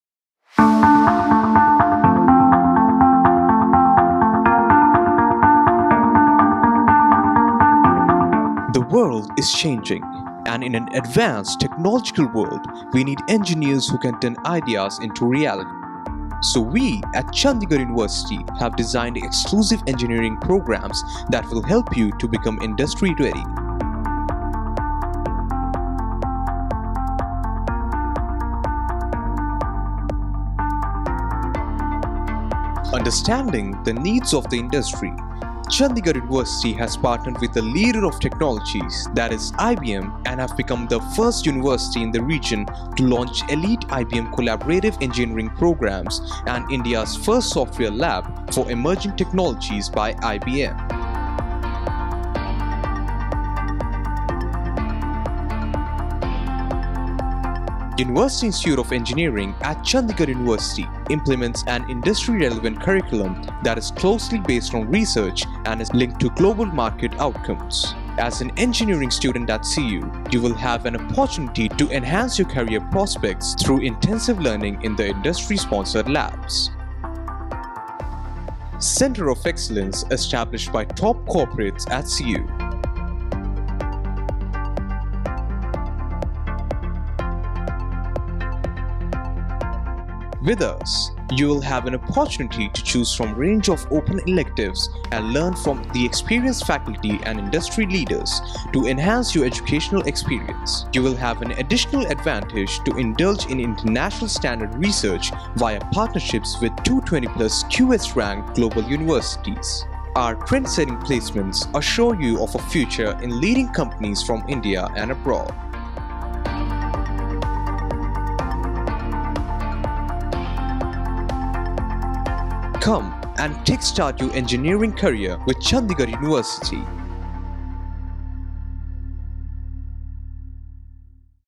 ENGINEERING at Chandigarh University
Preparing you for next-gen engineering & beyond at CU!

Looking for an engineering program that could make you industry-ready?

Join Chandigarh University’s exclusive engineering programs. From Industry-Relevant Curriculum –to- learning in Industry-Sponsored Labs, mentoring by Experienced Faculty and Industry Leaders –to- Unprecedented Placement Opportunities, you will be facilitated with everything to become next-generation, industry-ready engineer.

Admissions Open. Apply Now